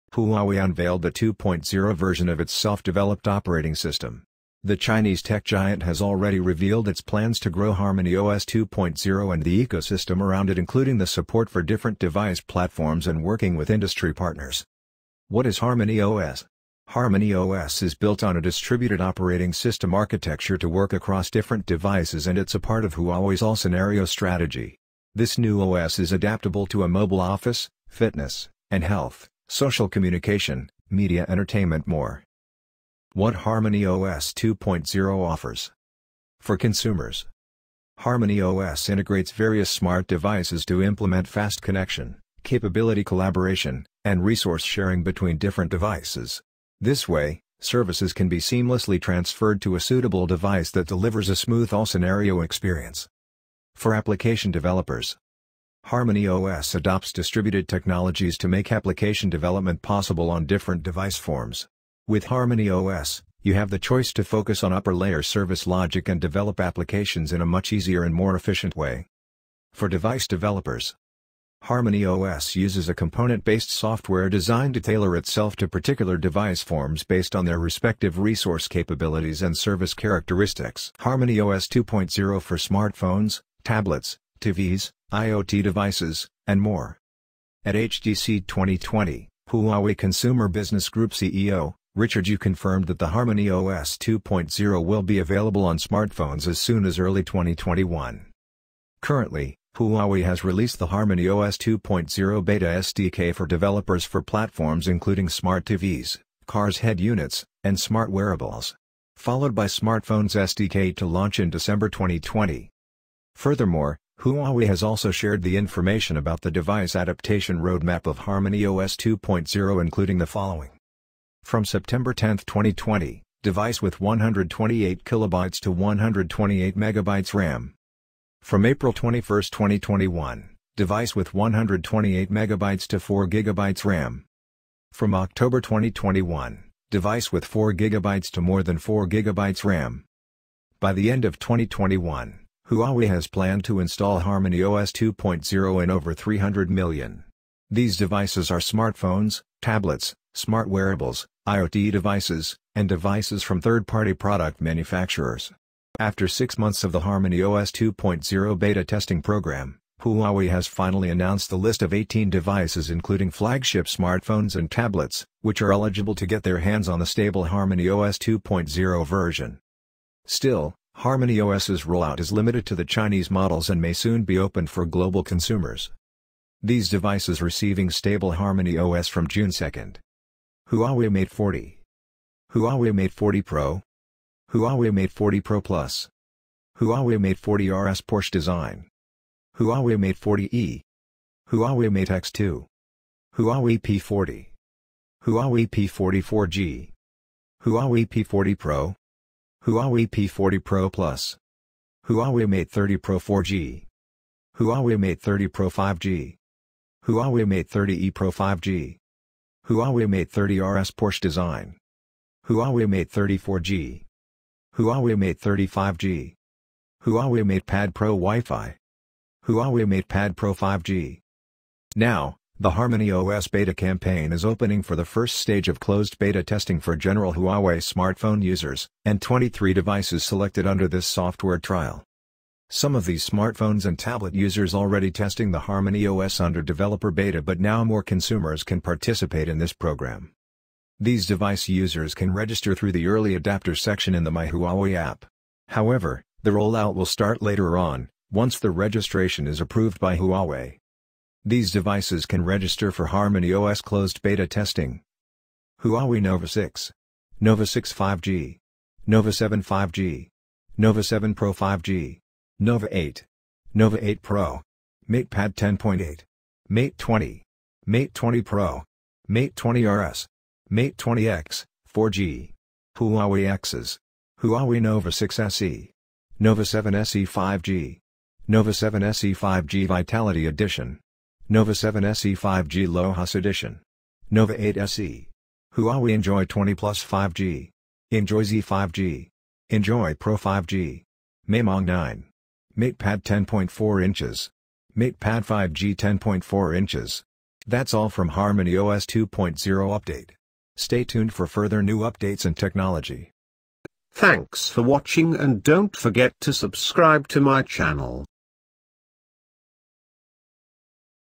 Harmony OS 2.0 - Why is it the Future of OS? Finally Android Competitor.
Harmony OS 2.0 fully explained also includes the mobiles compatible with Harmony OS 2.0 and every detail required to know about HarmonyOS 2.0.
Andriod's competitor is finally here. Harmony os 2.0 is finally the competitor of android.

Chapters:
0:00 HamonyOS Intro
0:34 What it offers?
1:40 Supported Devices
2:40  list of Mobile with stable HarmonyOS
4:10  List of Mobiles with Beta Version
4:56 END